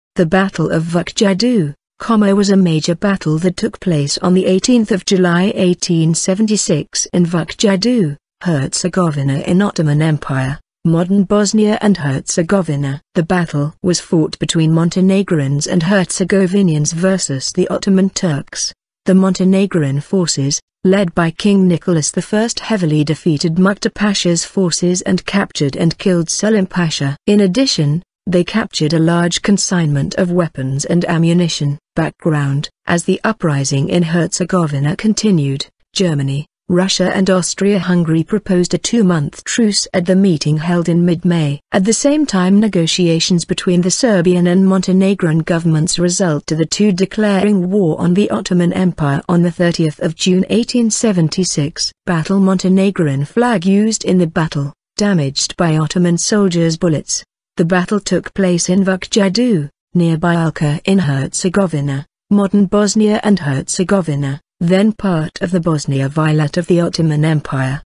The Ottoman Fought Battle of Vučji Do
The Battle of Vučji Do (Битка код Вучјег дола) was a major battle that took place on 18 July 1876 in Vučji Do, Herzegovina in Ottoman Empire (modern Bosnia and Herzegovina). The battle was fought between Montenegrins and Herzegovinians versus the Ottoman Turks. The Montenegrin forces, led by King Nicholas I heavily defeated Muktar Pasha's forces and captured and killed Selim Pasha. In addition, they captured a large consignment of weapons and ammunition.
Background

As the Uprising in Herzegovina continued, Germany, Russia and Austria-Hungary proposed a two-month truce at the meeting held in mid-May. At the same time negotiations between the Serbian and Montenegrin governments result to the two declaring war on the Ottoman Empire on 30 June 1876.
Battle
Montenegrin flag used in the battle, damaged by Ottoman soldiers' bullets.

The battle took place in Vučji Do, near Bileća in Herzegovina (modern Bosnia and Herzegovina), then part of the Bosnia Vilayet of the Ottoman Empire.